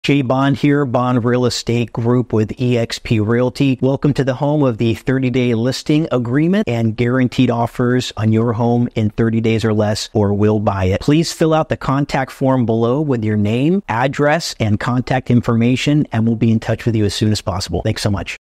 How to Generate Offers on your home in less than 30 days
Jay Bond discusses the Bond Real Estate Group's 30-day listing guarantee. Bond Real Estate Group will guarantee a 30-day listing term and offers on your home in less than 30 days or they will buy it.